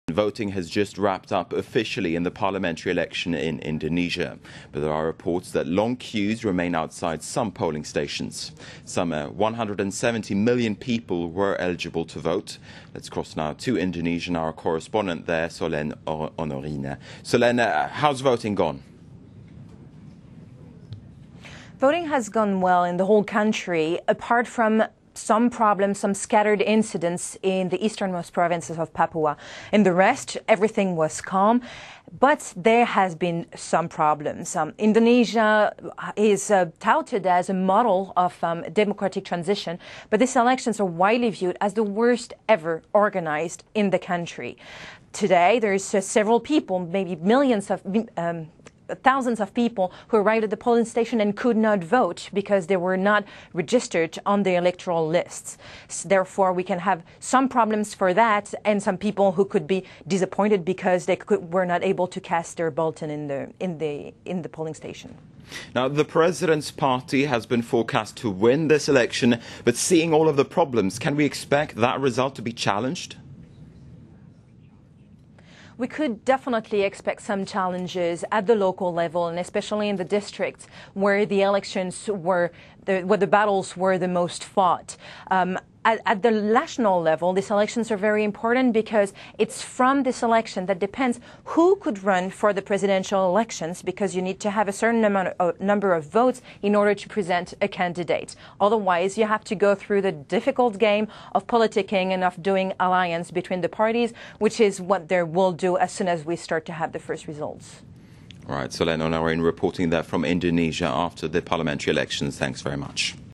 Indonesians vote in crucial general election
IN THE FIELD:  Indonesians voted in general elections to decide who can run for president in July.The Democrat Party is leading with 24.22 percent of the vote in the Papua region, according to a quick count by the National Survey Institute.